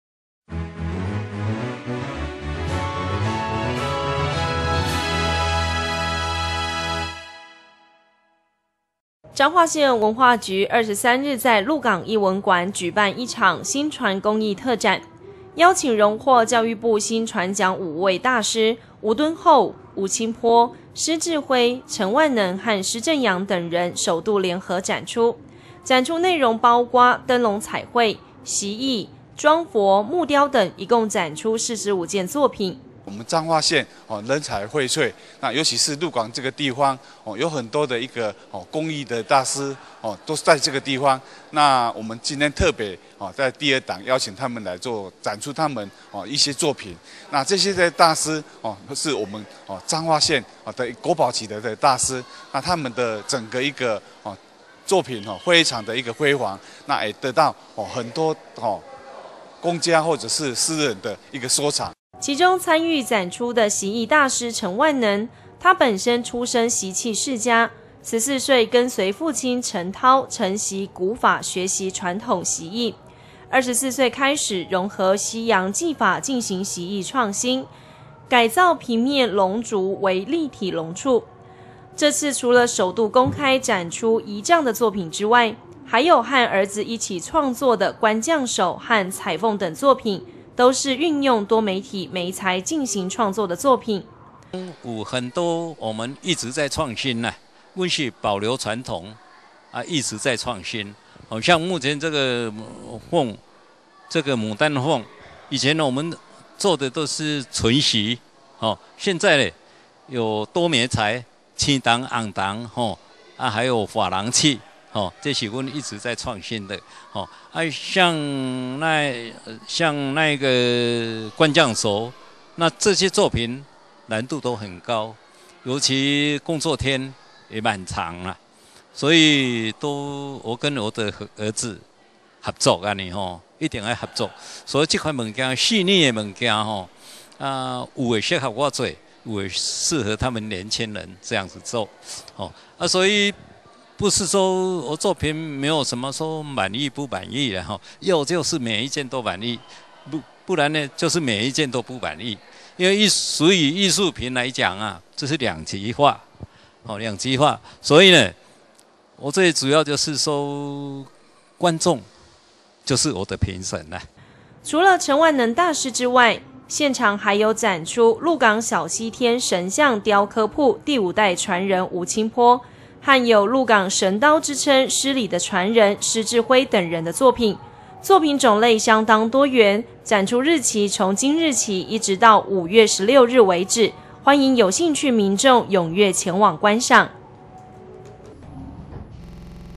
99-04-23 薪傳工藝特展 五位大師共同參展
彰化縣文化局是邀請榮獲教育部薪傳獎五位大師，吳敦厚、吳清波、施至輝、陳萬能和施鎮洋等十，首度在鹿港藝文館舉辦聯展。作品包括：燈籠彩繪、粧佛、錫藝、木雕等。部份作品都是第一次展出，相當珍貴，文化局歡迎鄉親踴躍前往參觀。